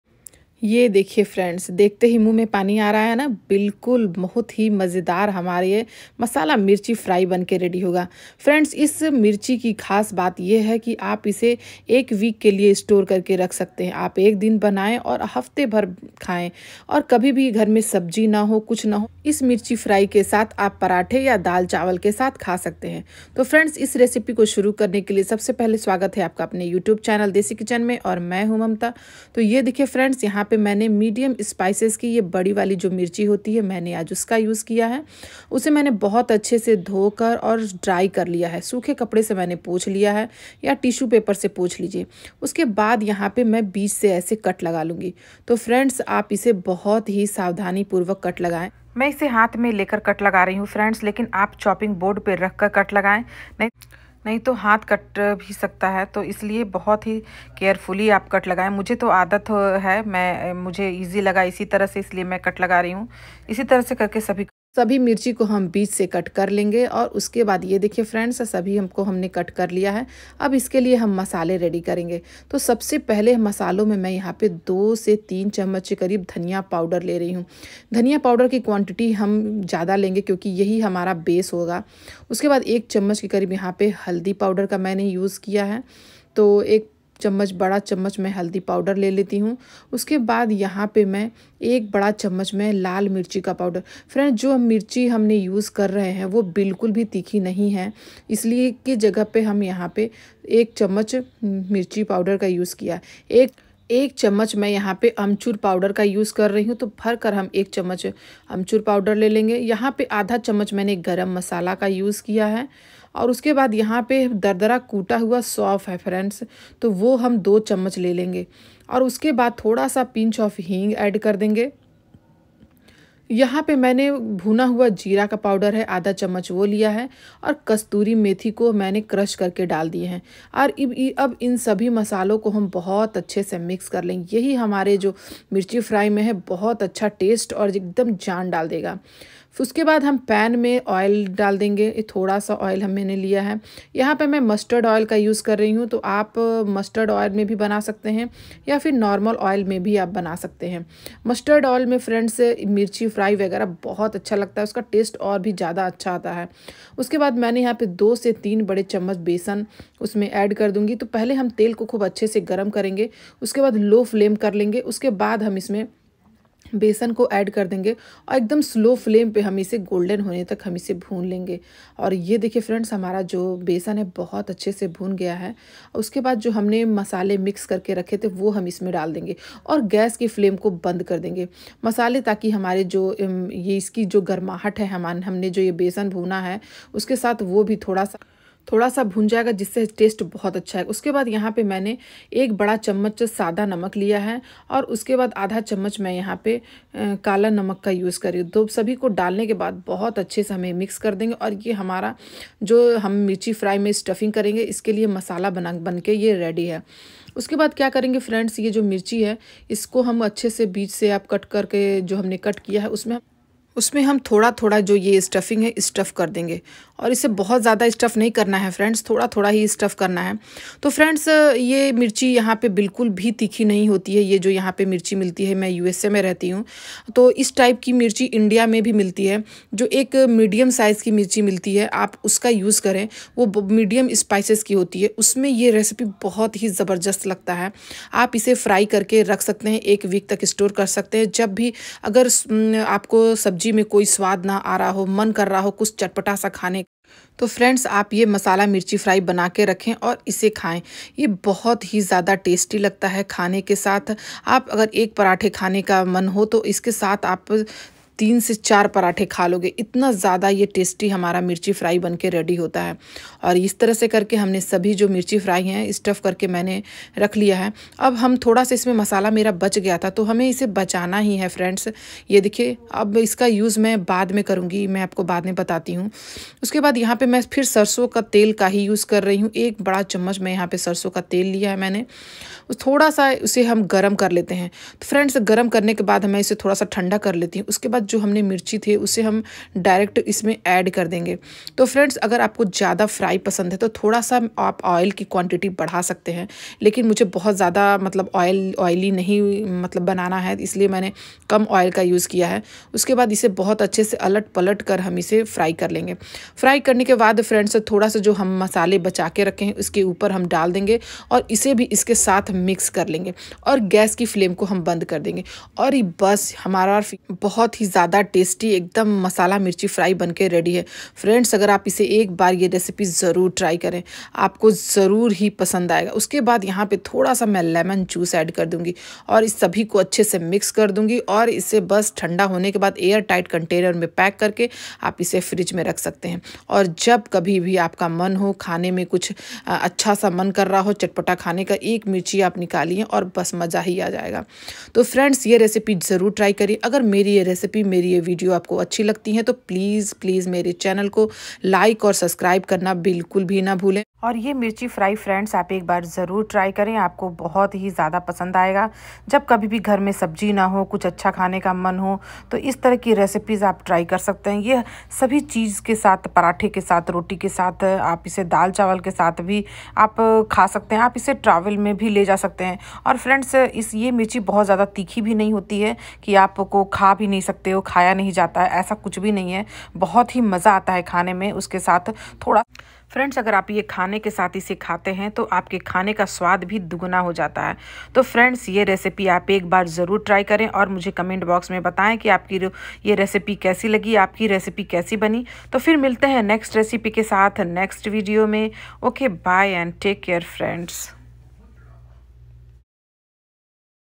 5 मिनट में बनाये चटपटी मसाला मिर्ची फ्राई स्वादिष्ट इतना की बिना भूख के 4 रोटी खा लेंगे ।mirchi fry
5 मिनट में बनाये चटपटी मसाला मिर्ची फ्राई स्वादिष्ट इतना की बिना भूख के 4 रोटी खा लेंगे ।mirchi fry#desikitchen